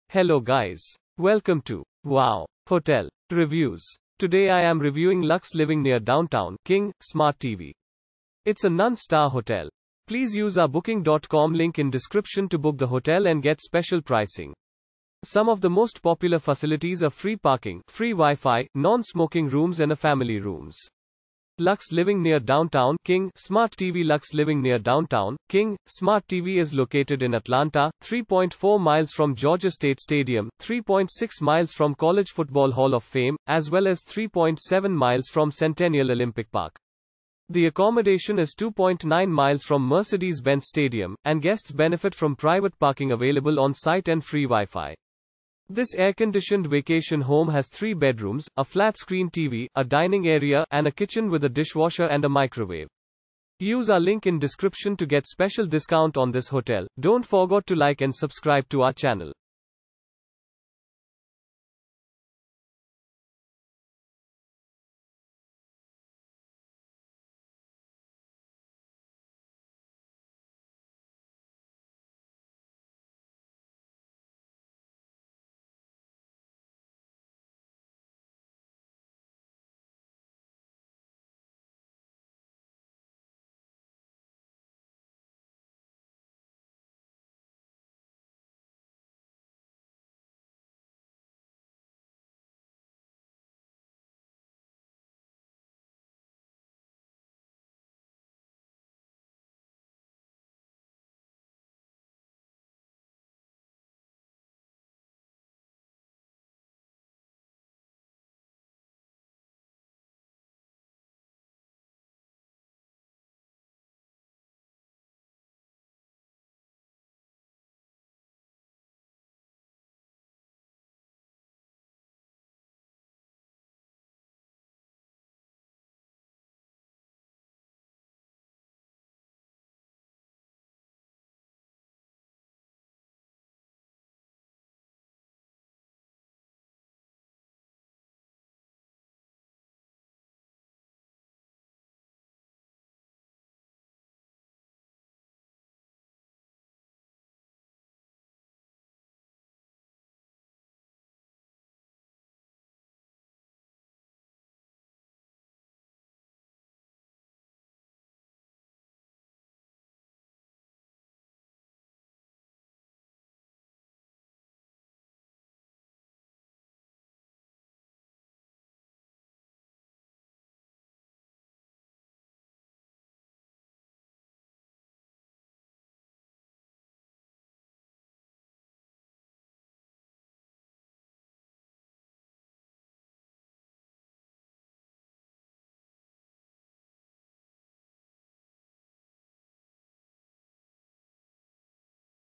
Luxe Living near Downtown, King,Smart TV Review - Atlanta , United States of America
Luxe Living near Downtown, King,Smart TV

Luxe Living near Downtown, King, Smart TV is located in Atlanta, 3.4 miles from Georgia State Stadium, 3.6 miles from College Football Hall of Fame, as well as 3.7 miles from Centennial Olympic Park. The accommodation is 2.9 miles from Mercedes-Benz Stadium, and guests benefit from private parking available on site and free WiFi.
This air-conditioned vacation home has 3 bedrooms, a flat-screen TV, a dining area, and a kitchen with a dishwasher and a microwave.